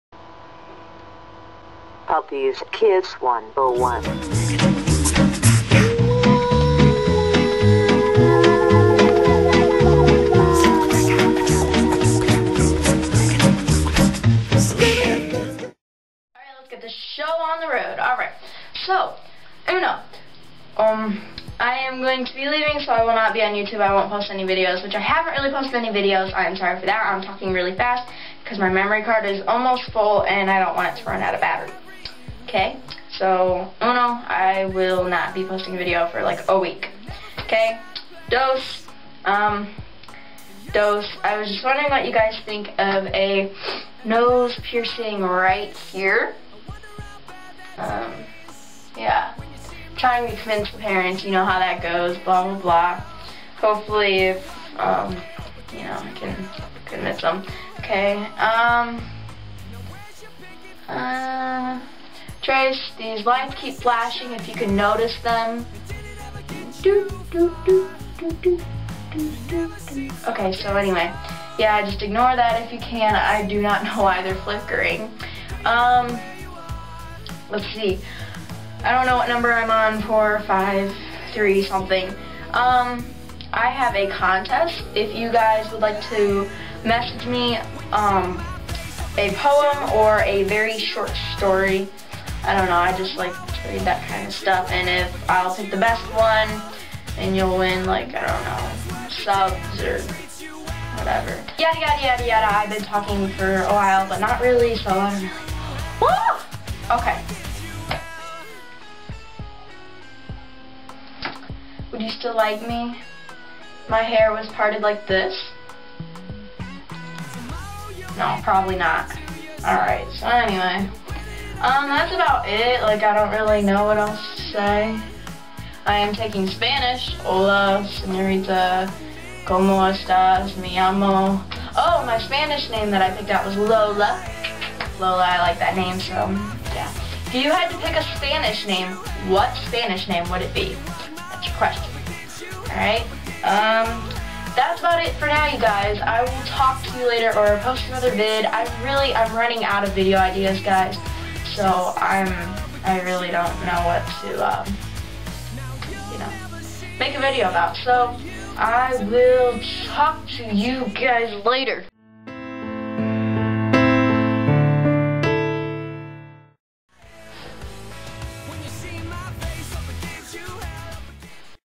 A New Video OMG!!! [Important Information]
hey guys, update video again! just lettin you know whats goin on, if you want, send me a poem or short story, i will read, pick the one i like best, and uhm,,,, idk give you some subs or comment your videos or something like that, lol. tell me your favorite spanish name if youre already spanish, im sorry, lol. u guys rock!